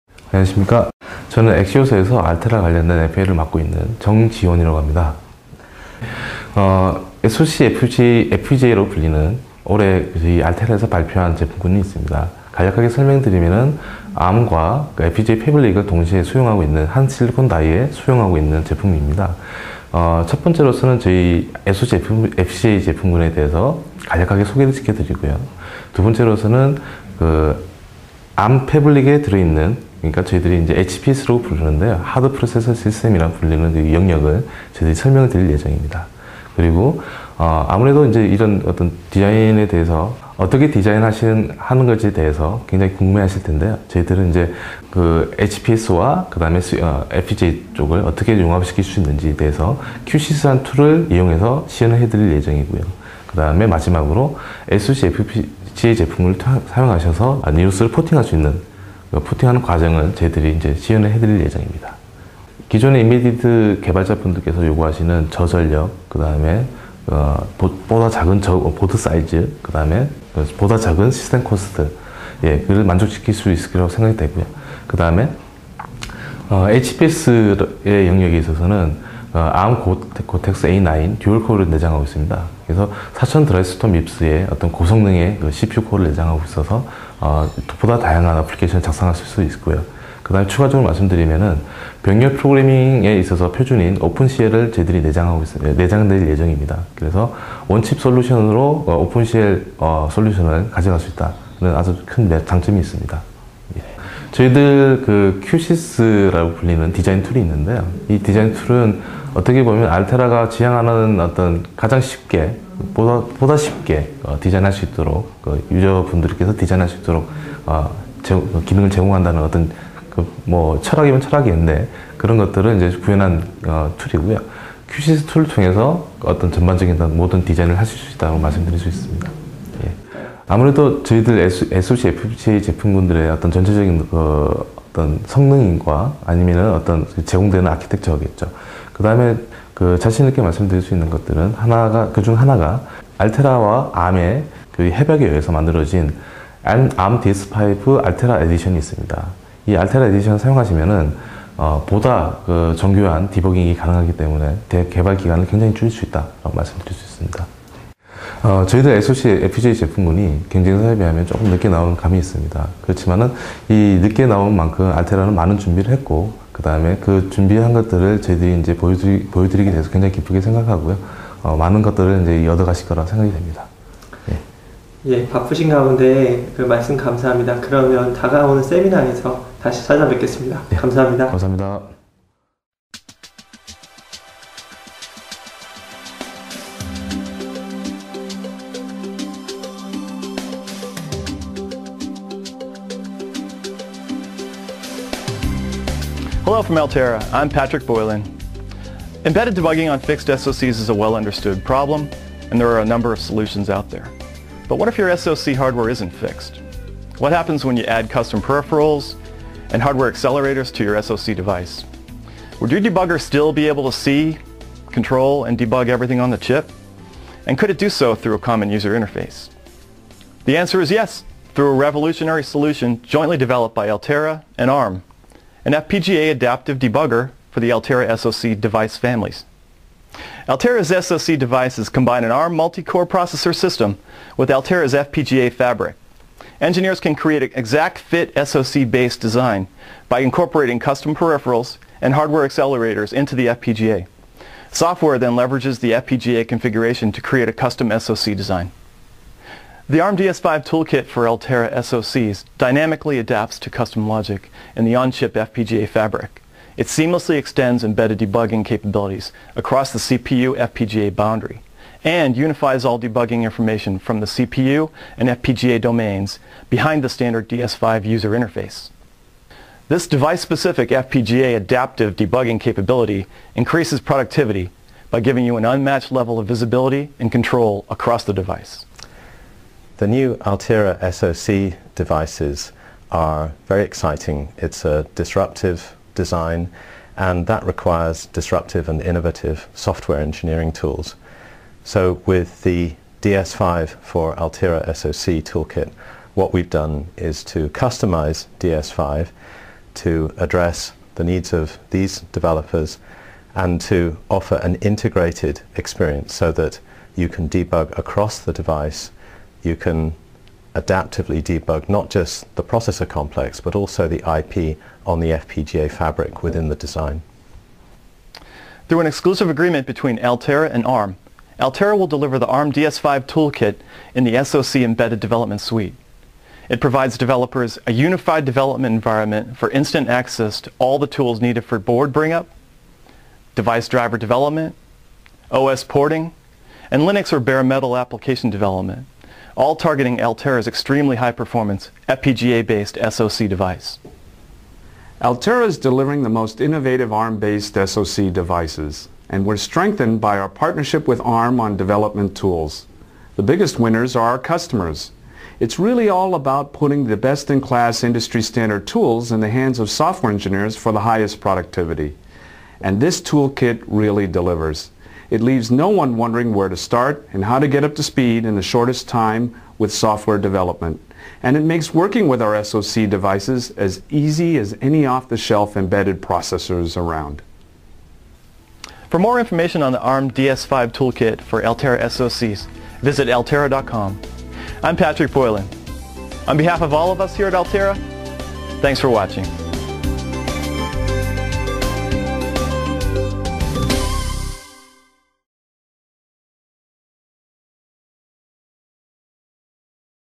SoC FPGA로 고성능 임베디드 시스템 설계 쉽게 구축하기
SoC FPGA는 프로세서, 주변기기 및 메모리 인터페이스로 구성된 ARM ® 기반 하드 프로세서 시스템 (HPS)을 고 대역폭 인터페이스 커넥터 백본에 FPGA 패브릭과 통합 한 제품입니다. 이렇게하면 하드 IP의 성능과 소비 전력의 절감과 프로그래머블 로직의 유연성이라는 장점을 겸비합니다.
이 웹캐스트에서는
쉽고 유연한 설계툴인 Qsys를 이용하여 H/W를 설계하고 ARM DS-5 Altera edition을 사용하여 보다 빨리 S/W를 개발하는 과정을 시연할 예정입니다.
1. SoC FPGA 개요
2. HPS(Hard Processor System) 구조
3. Qsys로 SoC FPGA 설계
4. SoC FPGA에 Linux 포팅과정 시연.
5. Q&A : 온라인 실시간